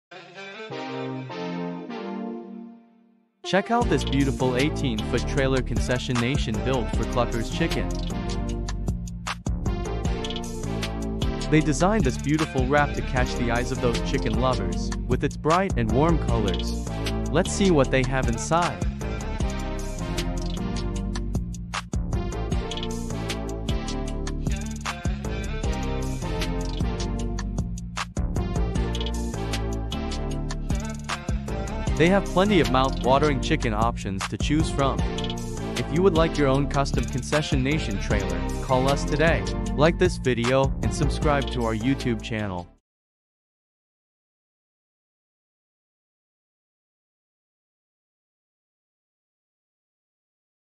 Cluckerz Chicken | 8x18 Concession Trailer | Concession Nation
Beautiful 8x18 concession trailer built for Cluckerz Chicken. Are you looking for concession trailers for sale?